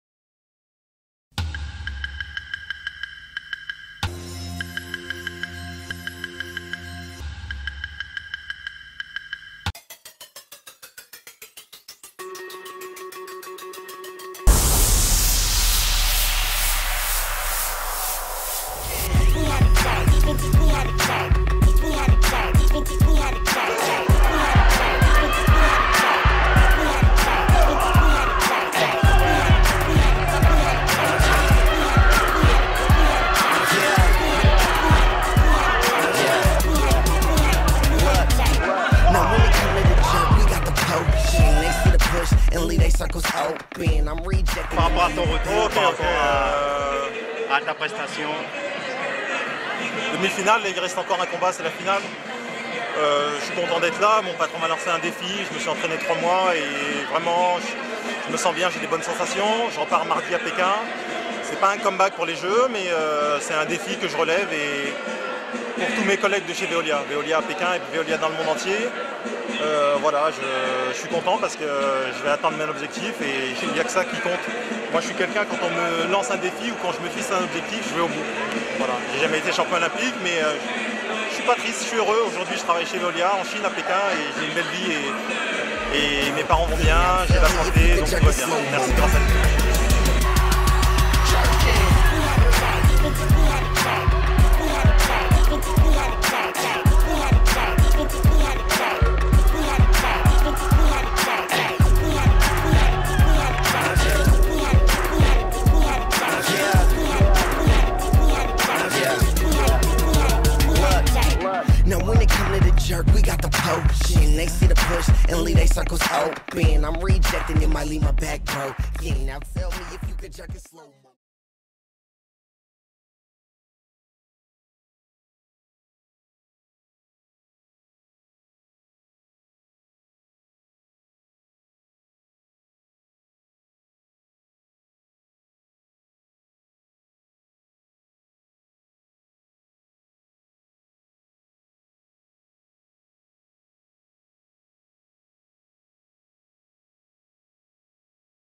Pascal Gentil.mov
Pascal Gentil, à deux reprises médaillées de bronze olympique a signé un retour gagnant, enfin je voulais dire un quinzième retour gagnant ce samedi 5 février 2011 en remportant une fois de plus sa finale 8-1 contre Mathias Maizeroi au championnat de France de taekwondo à Strasbourg.
A 37 ans, le tout jeune retraité des tatamis suscitait la curiosité de tous à savoir s'il se préparait pour les prochains j-o de Londres en 2012.
 L'icône du tkd français nous à confié qu'il était juste là pour relever un défit, et qu'aujourd'hui, seul son avenir professionnel au sein de son entreprise Veolia Pékin l'intéressait...
Beau retour  gagnant mais non moins éphémère de cet athlète hors du commun qui aura marqué son époque et qui aura  écrit quelques pages de l'histoire du taekwondo français.

Droit de reproduction et de diffusion réservés-©-LS-PRODUCTION